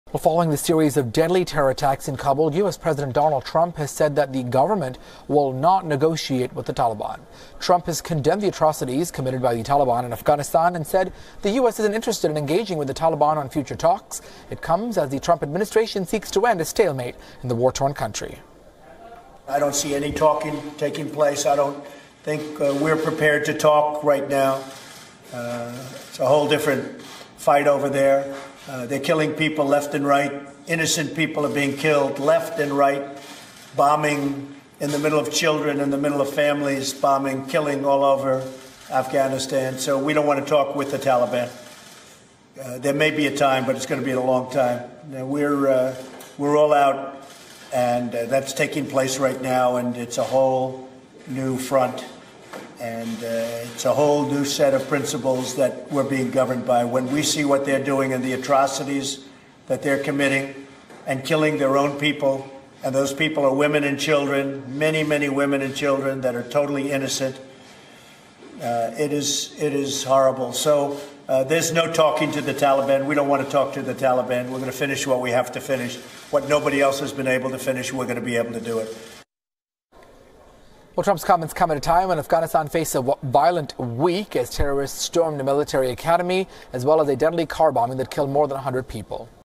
Trump rejects idea of talk with Taliban
Trump: No talks with Taliban; condemns terror attack in Kabul. Watch the video to know more about the news.